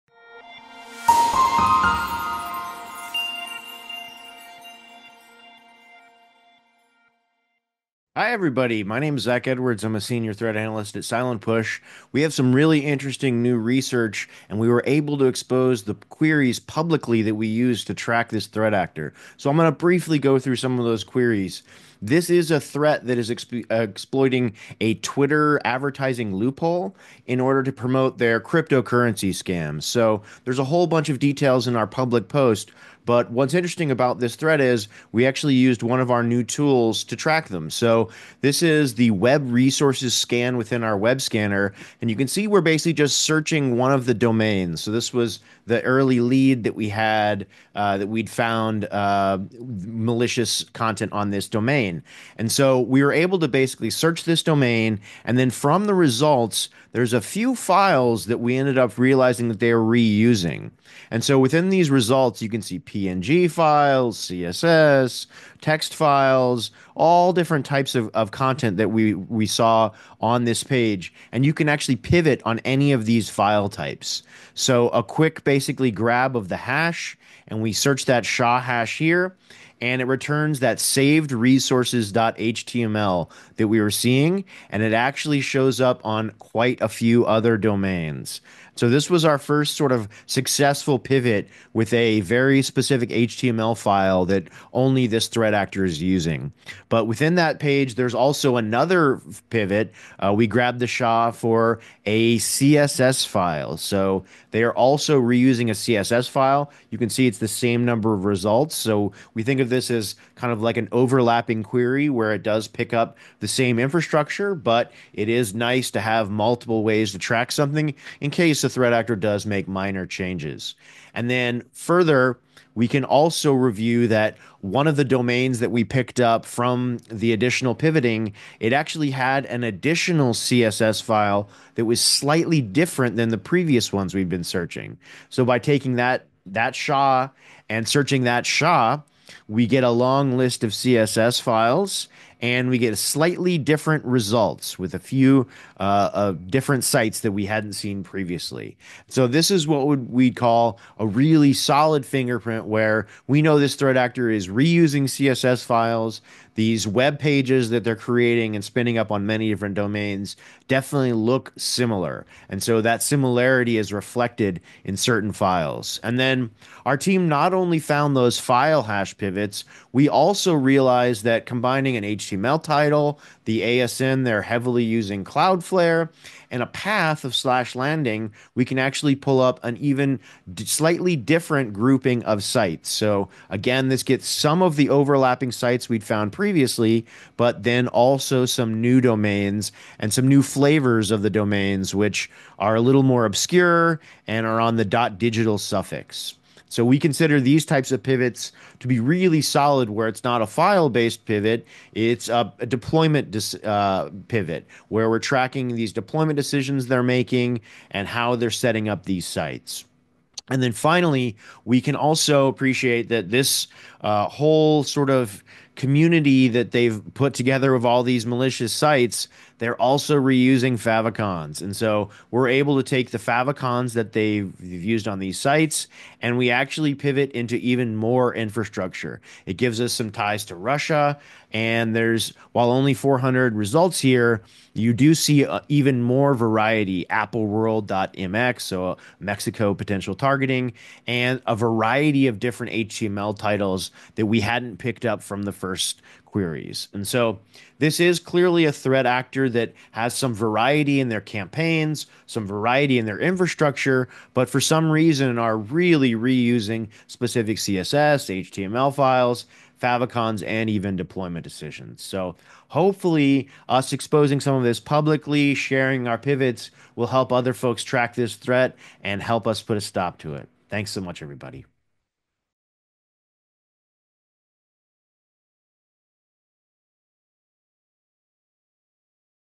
How to Detect Threat Actor Infrastructure with Web Resource Scan
Silent Push Senior Threat Researcher Zach Edwards takes you through how to use Web Resource Scan to detect and fingerprint attacker infrastructure.